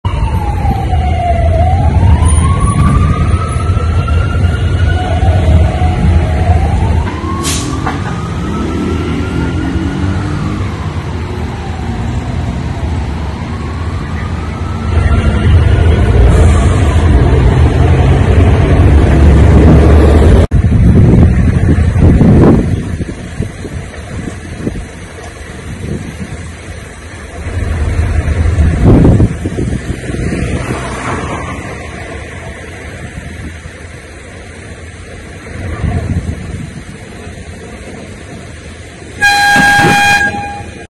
Un hombre fue atacado a balazos afuera de la sucursal de Scotiabank de Plaza Dorada
Un hombre fue atacado a balazos afuera de la sucursal de Scotiabank de Plaza Dorada, lo que generó una fuerte movilización policíaca